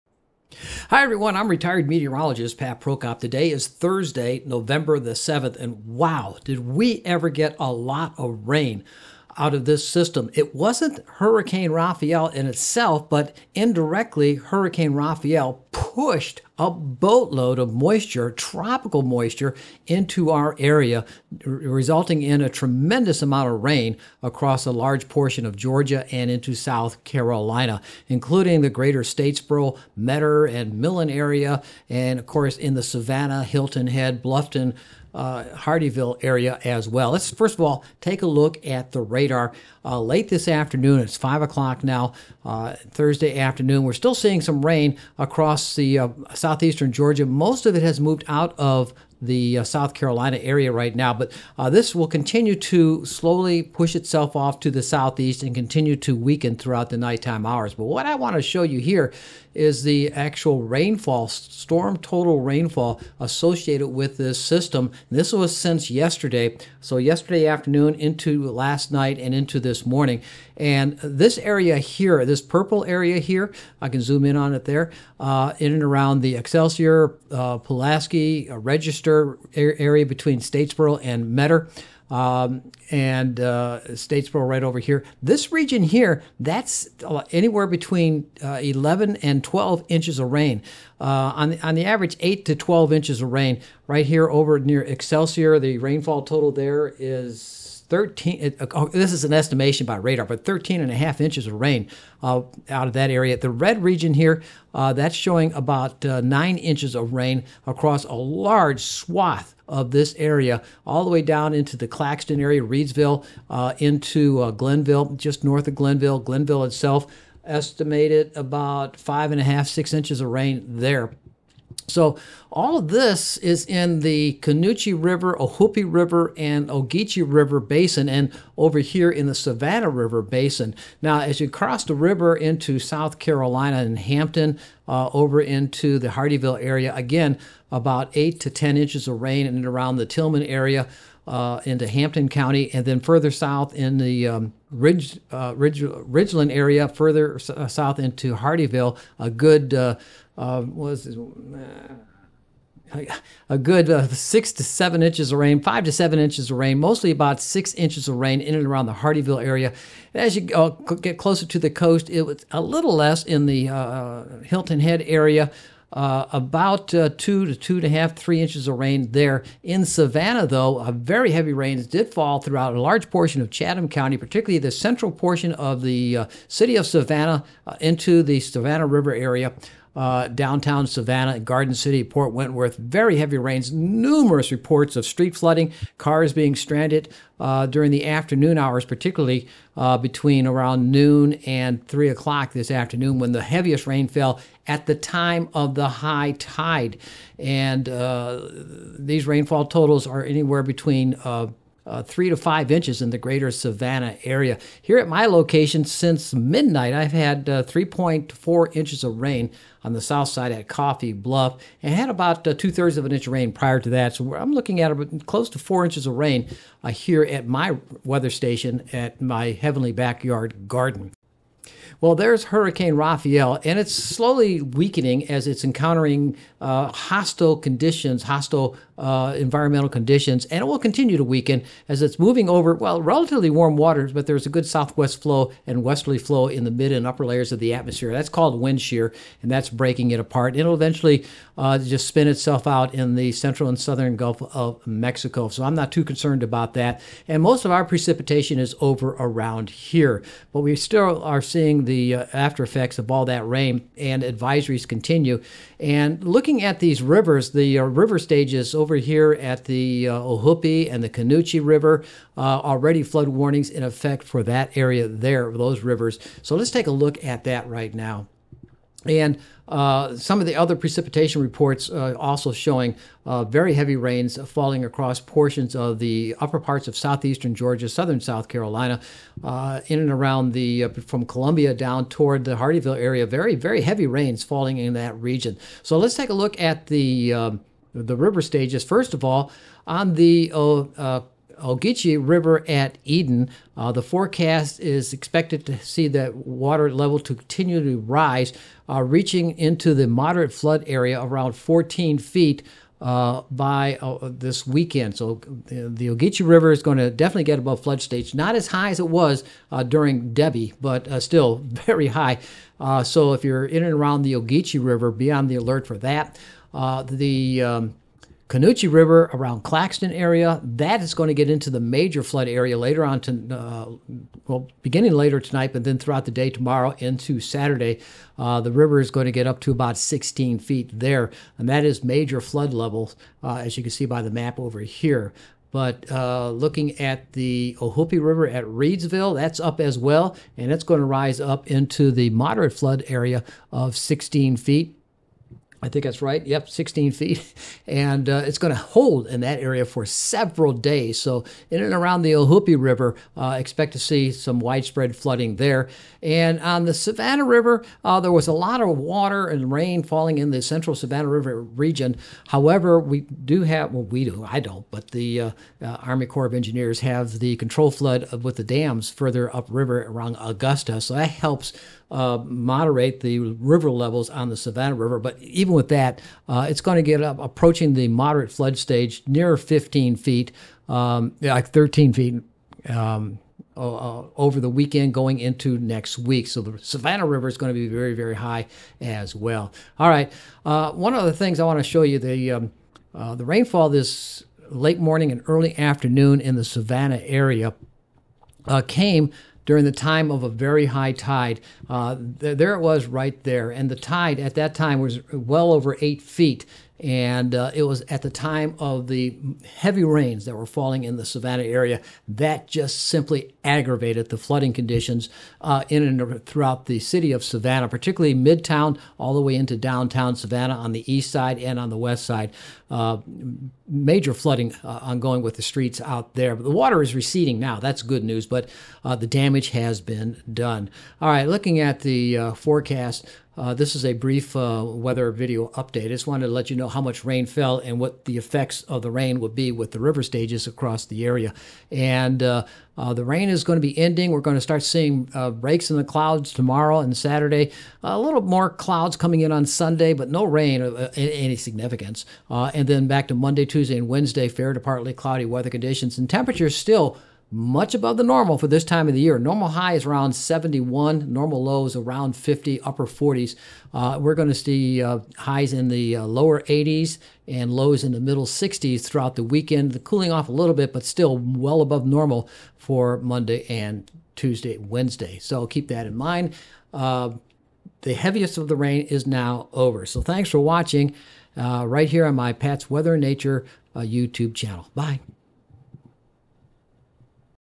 Weather with Pat Prokop Nov 7 Heavy Rain Effect
Very Heavy Rains fell over our region overnight and most of today.  How Much and how will it affect the rivers?

or
You can buy me a cup of coffee for only $2 a cup (I love coffee BTW) at